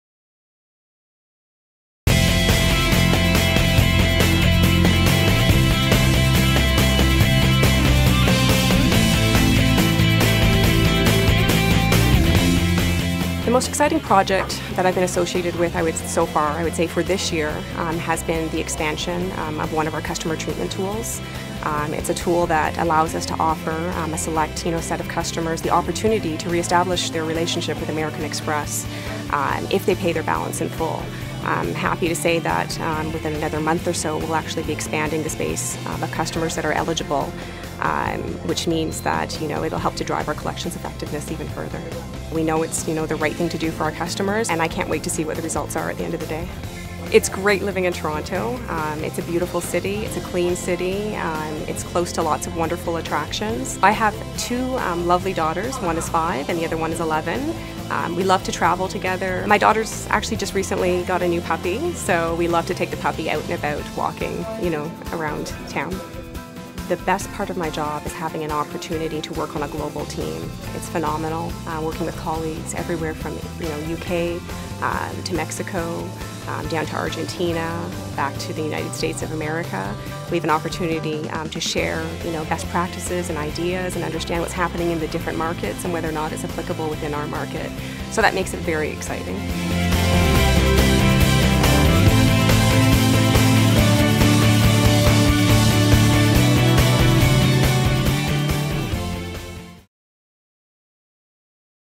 Collections Management Careers foraliving -- American Express
See Leah describe her career working at American Express in Toronto within Global Creadit Administration.